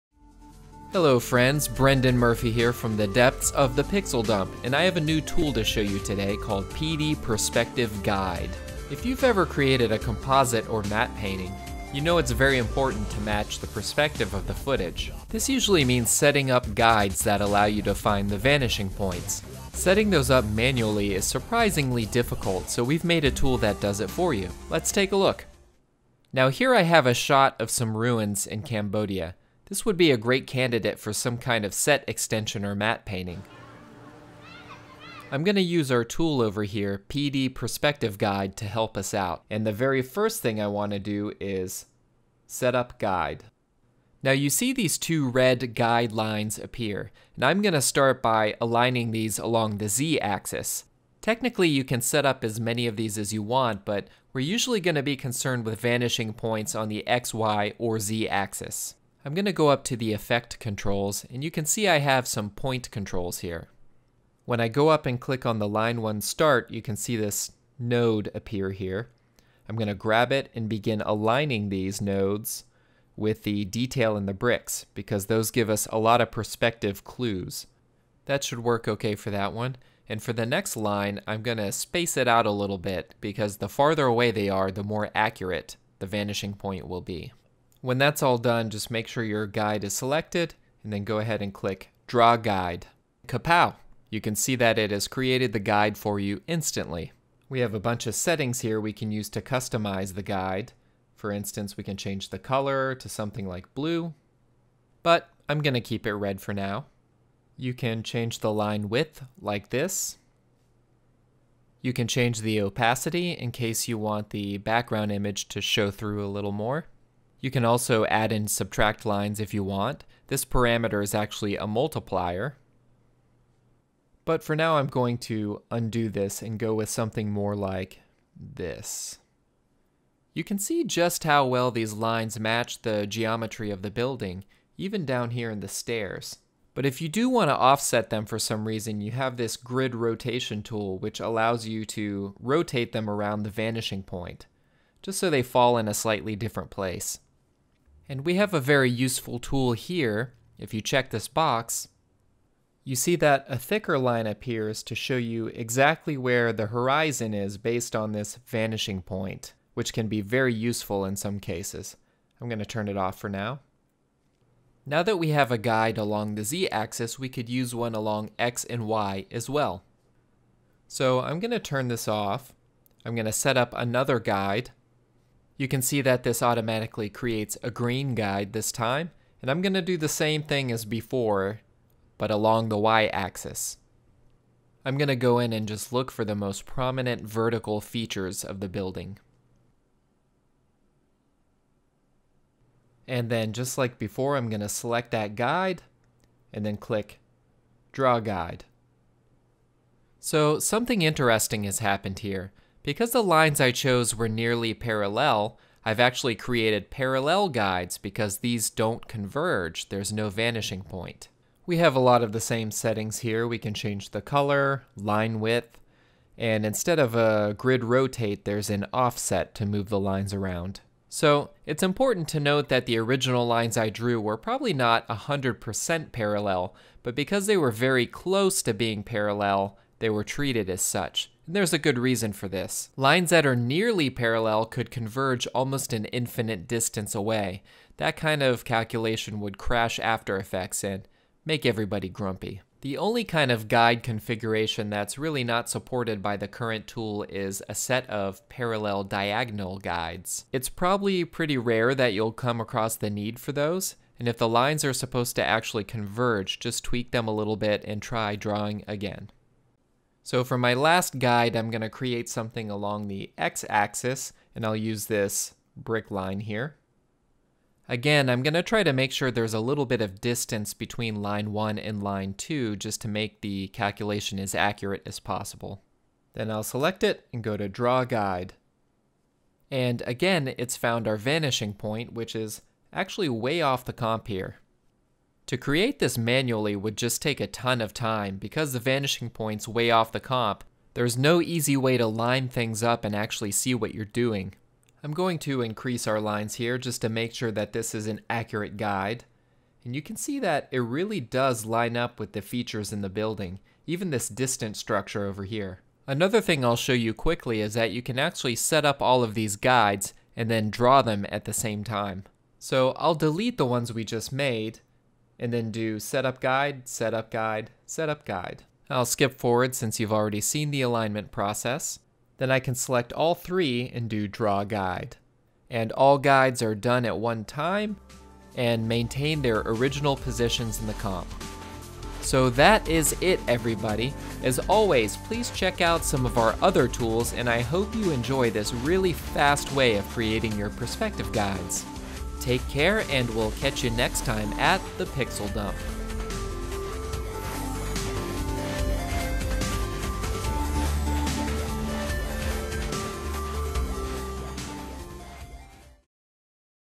PD Perspective Guide
Making perspective guides in AE is a pain. NOT ANYMORE!  Set a couple points and this tool does the rest!  **Now working with CC 2024